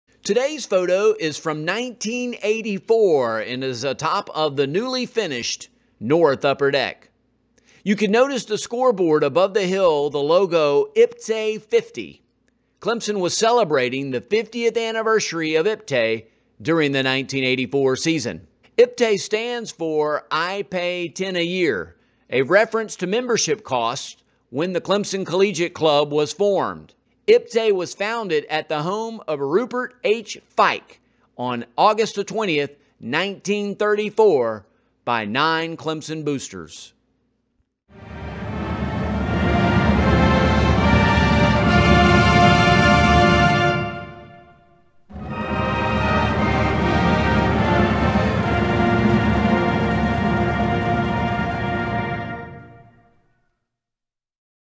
June 24th Clemson Historic Picture Of The Day
Today's photo is from 1984 and is atop the newly finished North Upper Deck. You can notice on the scoreboard above the hill the logo IPTAY 50.

Clemson was celebrating the 50th Anniversary of IPTAY during the 1984 season

IPTAY stands for I Pay Ten A Year, a reference to membership costs when the Clemson collegiate club was formed. IPTAY was founded at the home of Rupert H. Fike, M.D., on August 20, 1934, by nine Clemson boosters.